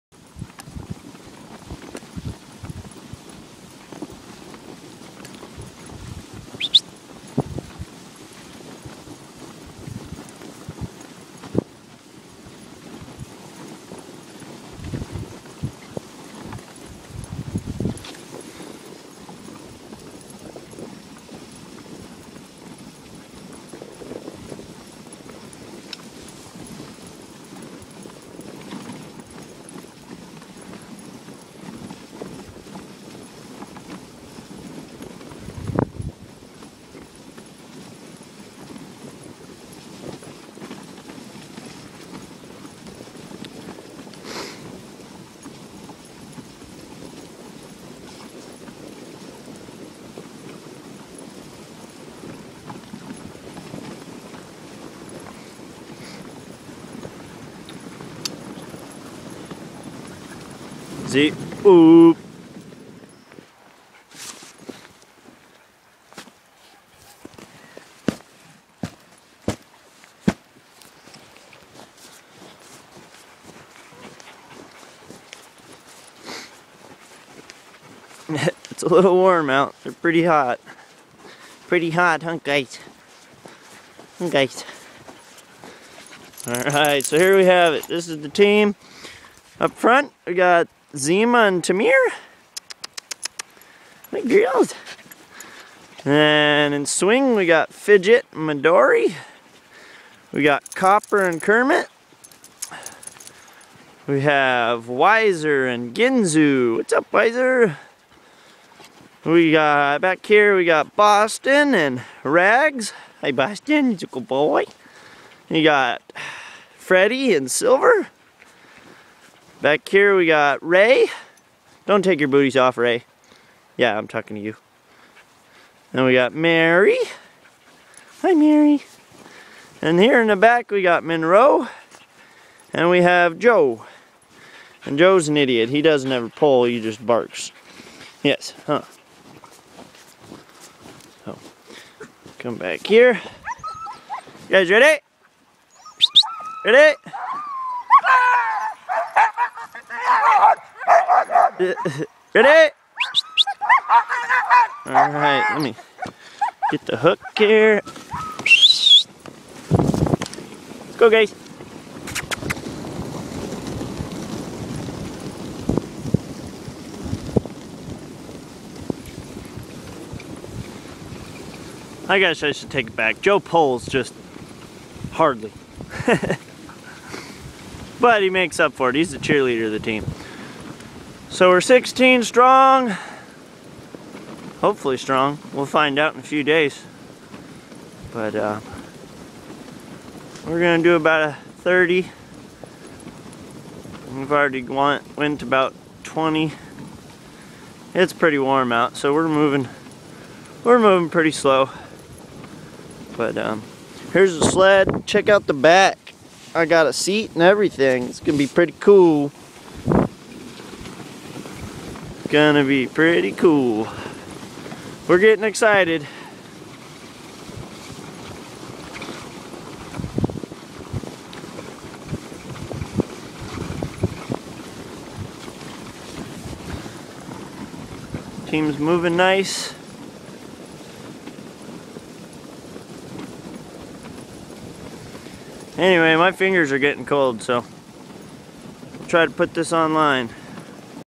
2013 Iditarod Team in Action
2013 Iditarod rookie Travis Beals of Seward, Alaska and Turning Heads Kennel introduces his all star Iditarod team.